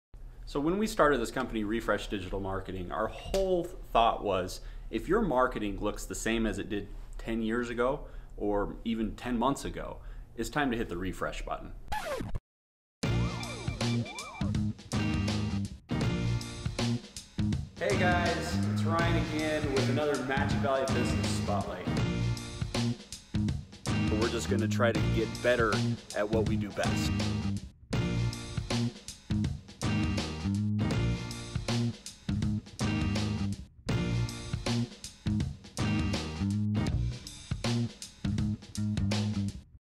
Refresh Introduction Video
Hi everyone, Refresh here! We are a digital marketing company that specializes in getting real results, for real people. We'd love to share our journey with you, as well as stories from local business owners in the Magic Valley, and what's new in the digital marketing world! Thanks!